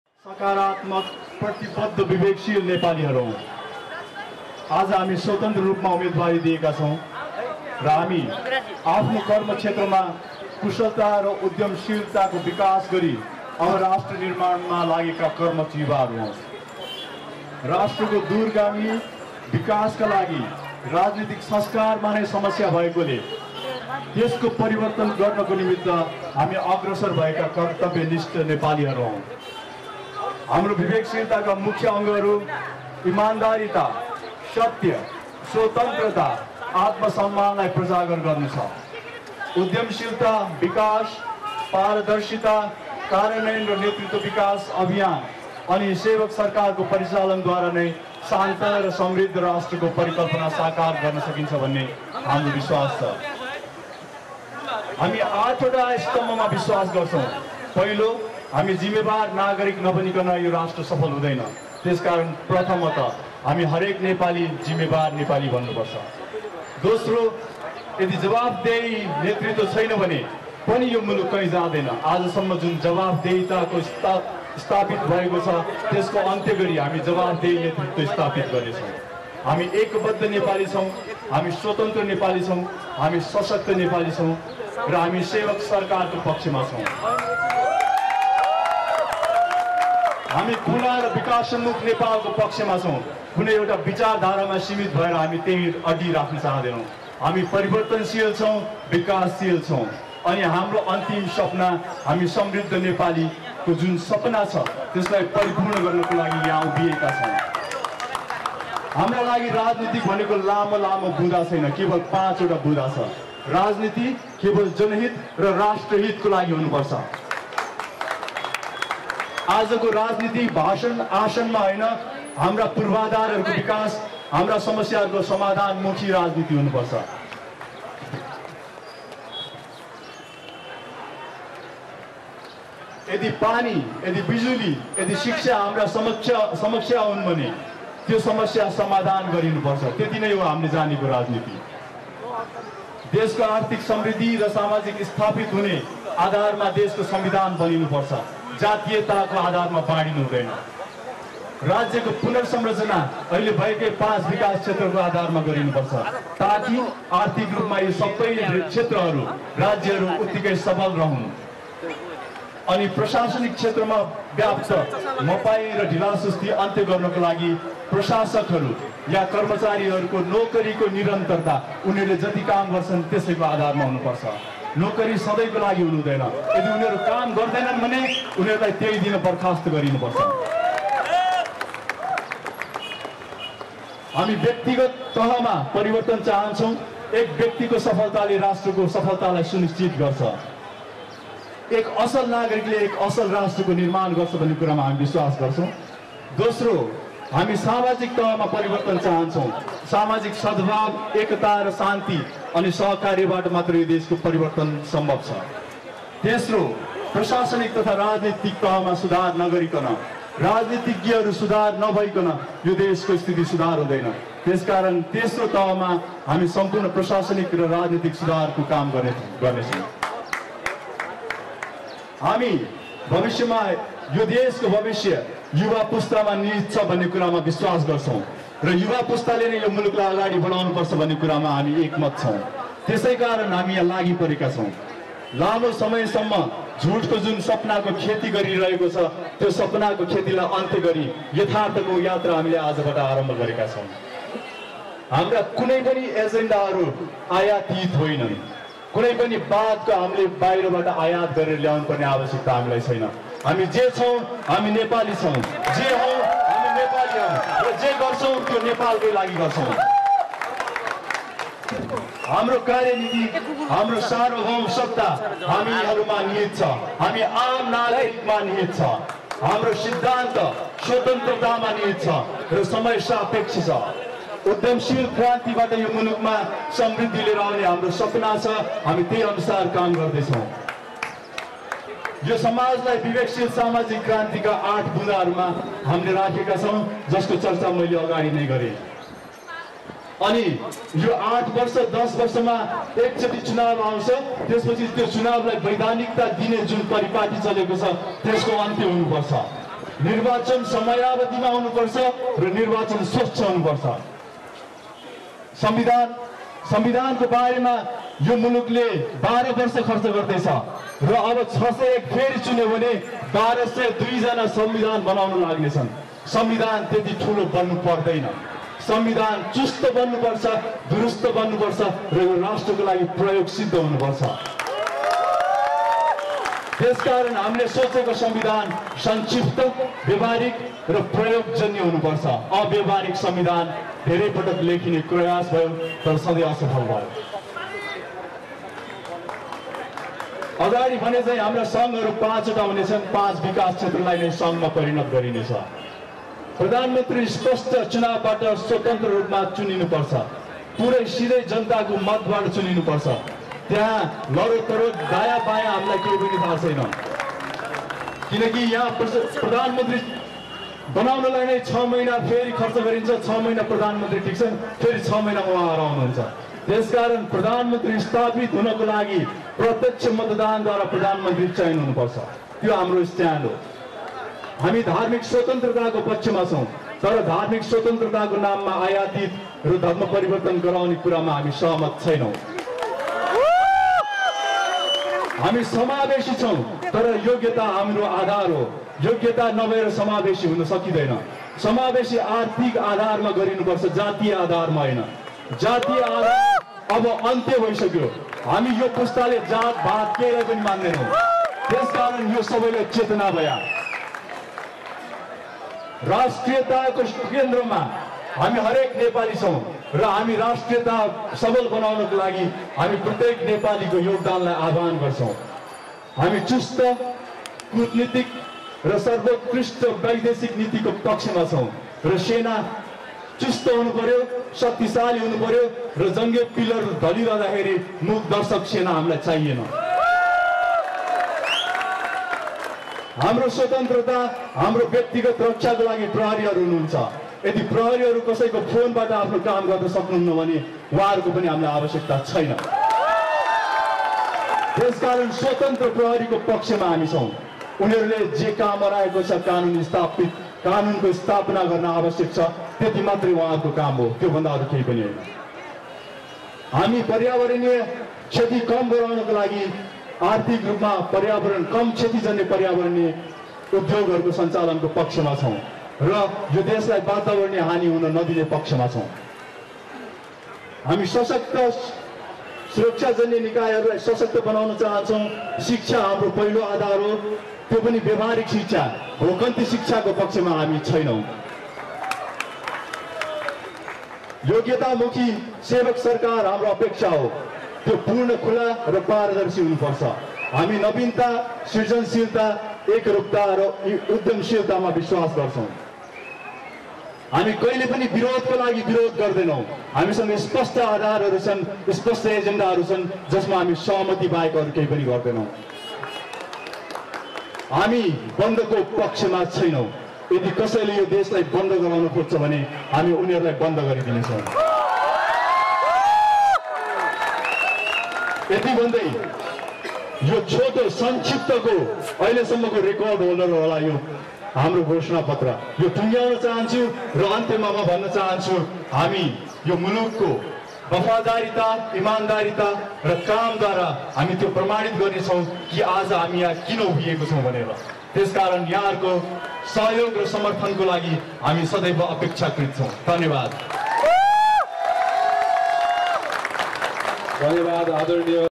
विवेकशील नेपाली घोषणापत्र
बिबेकशील नेपाली को छोटो मिठो घोषणापत्र बचन गर्दै बिबेकशील युवा उद्यमी नेता गोविन्द नारायण  नेपाल हाम्रो घर हो, डेरा होईन। आउनुहोस् विवेकशील नेपाली बनौँ, नेपाली राजनितिलाई सुसंस्कृत बनाऔँ ! अब बिकल्प कुर्दै बस्ने होइन आफै हुने हो!